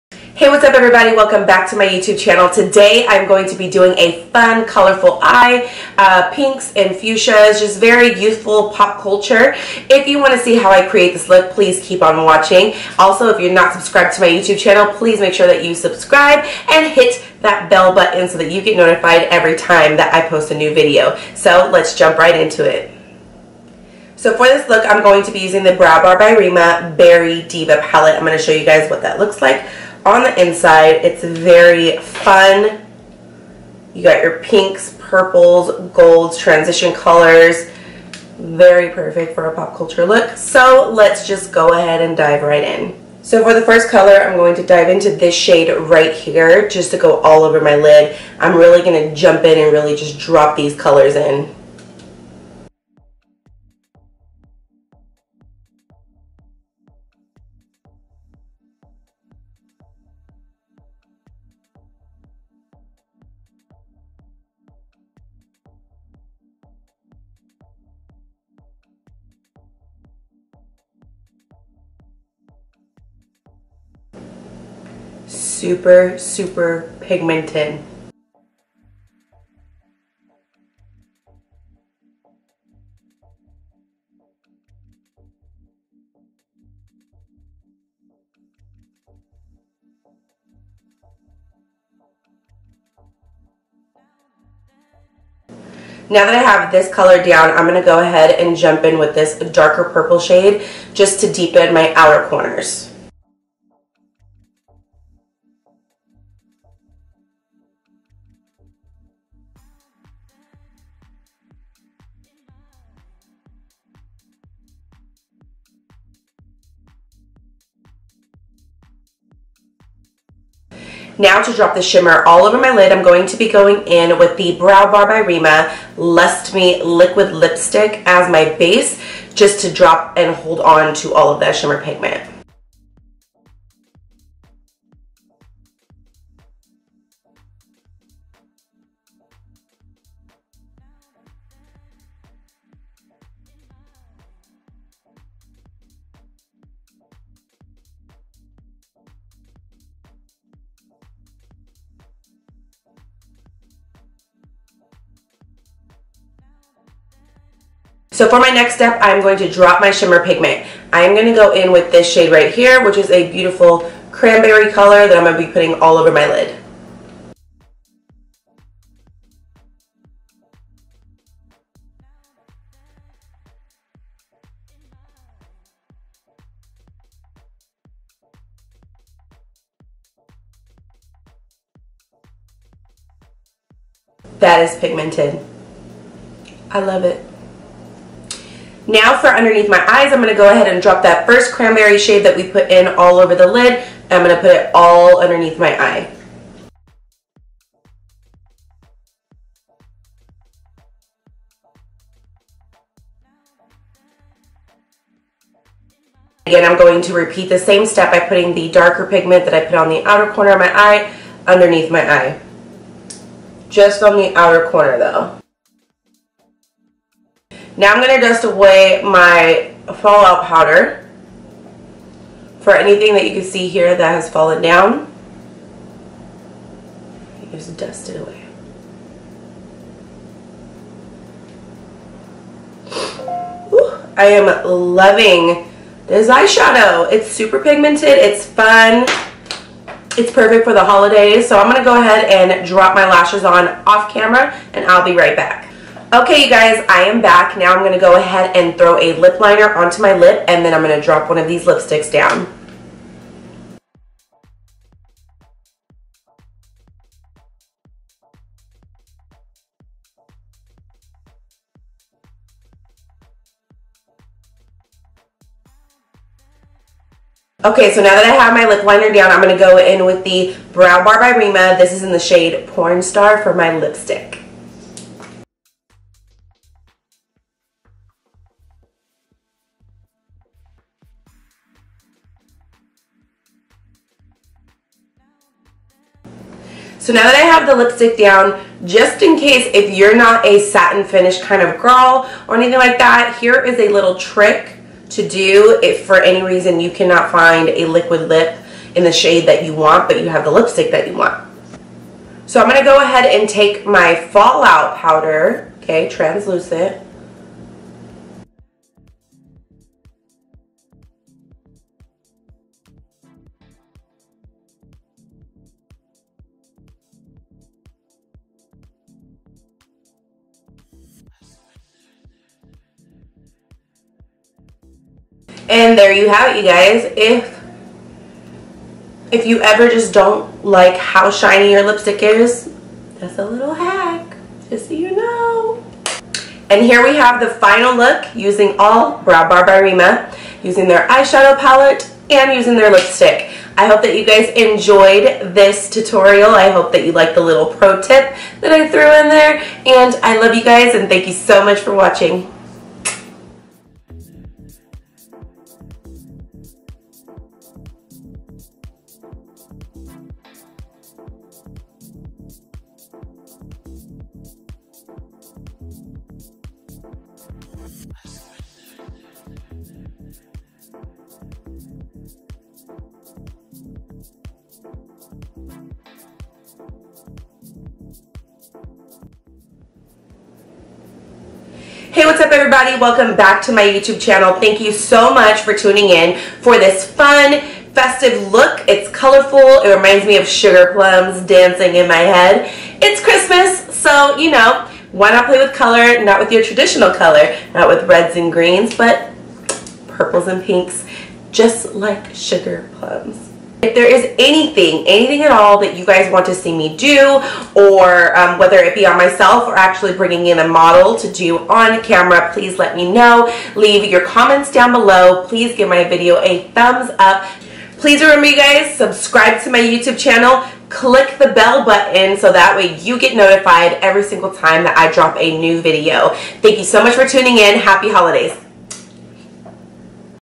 HOLIDAY GLAM TUTORIAL | TheNicholeRay
This is a look using all "Brow Bar By Reema"

PRODUCTS USED:

- All products are Brow Bar By Reema